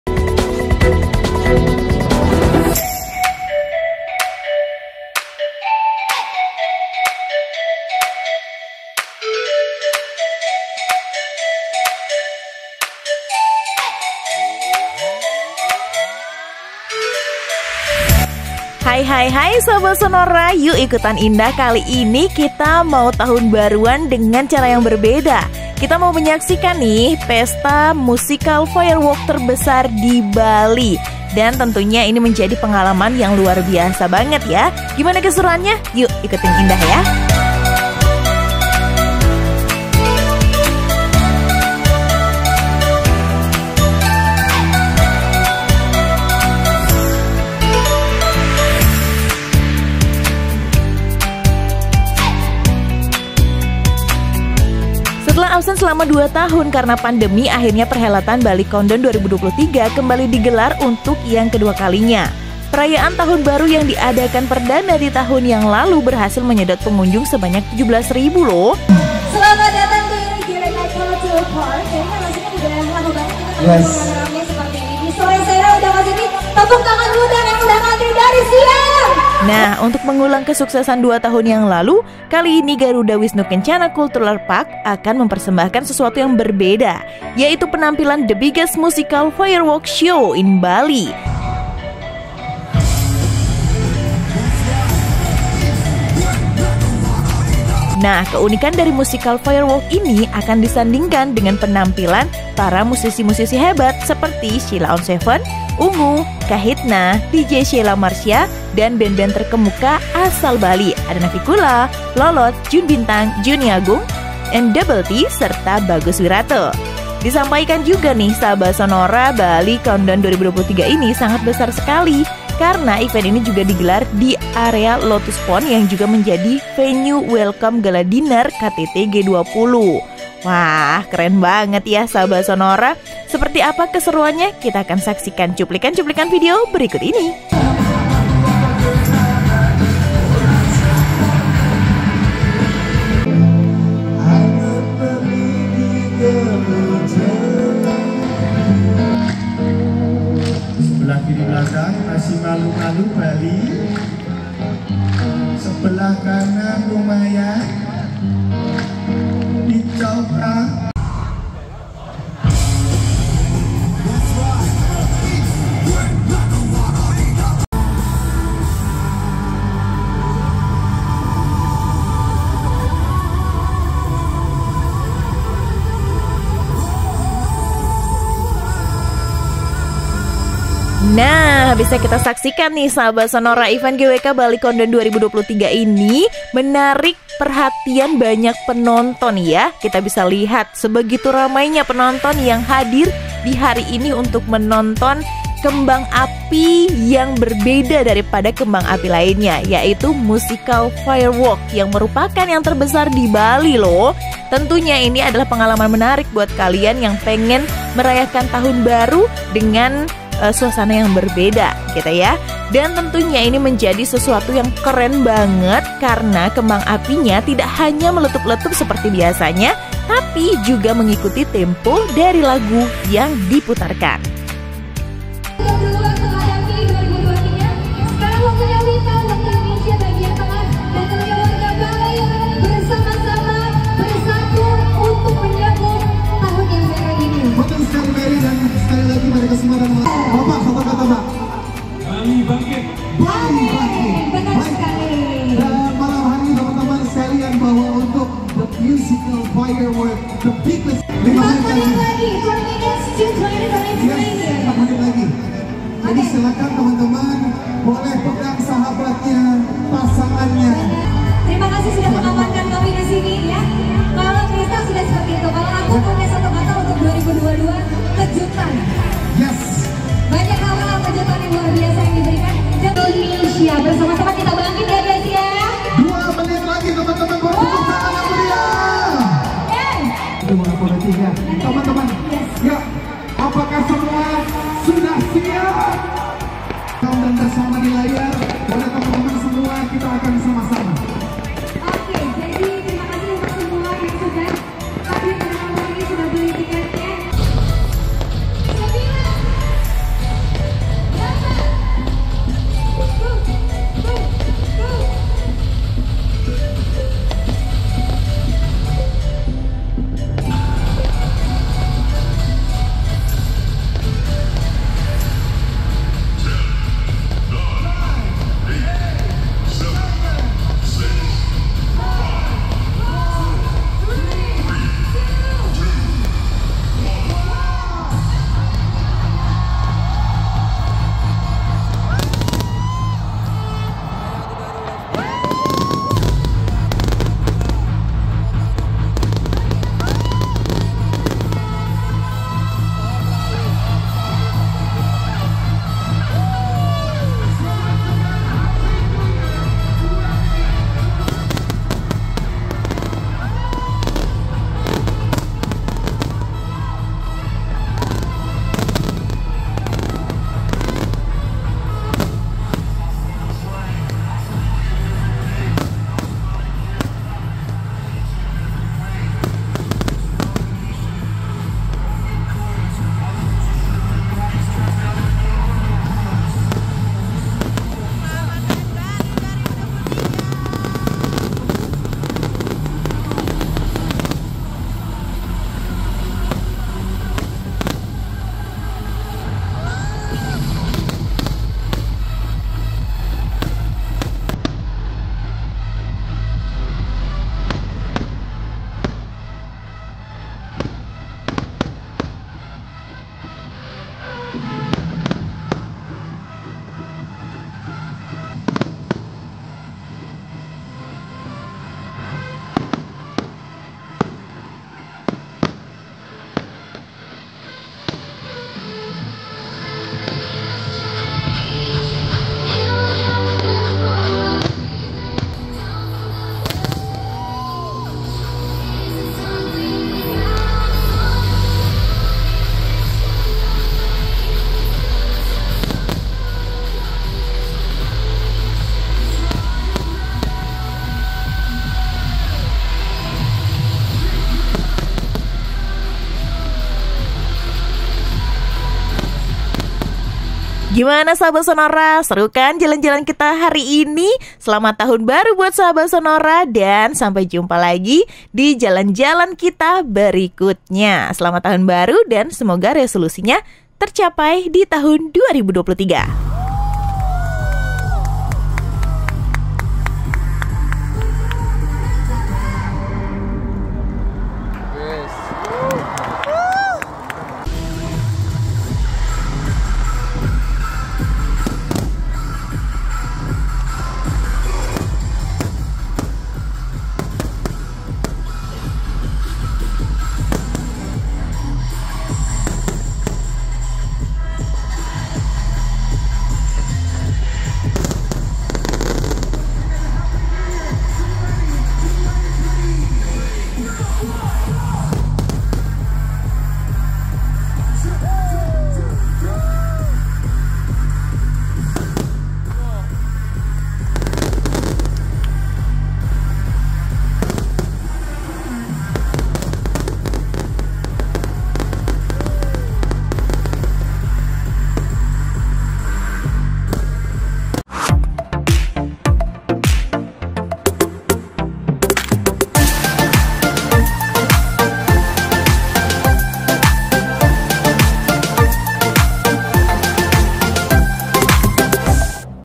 TAHUN BARU DI BALI 2023 FENOMENAL ! KEMBANG API UNIK TERBESAR PERTAMA
Kawasan Garuda Wisnu Kencana (GWK)  menjadi salah satu pusat perayaan malam tahun baru 2023 di Bali. Sederet musisi papan atas seperti Sheila On7 hingga Kahitna akan meramaikan pesta tahun baru dalam acara bertajuk Bali Countdown 2023.
Acara yang digelar selama dua hari (31 Desember 2022 dan 1 Januari 2023) itu diklaim sebagai the biggest musical fireworks in Bali.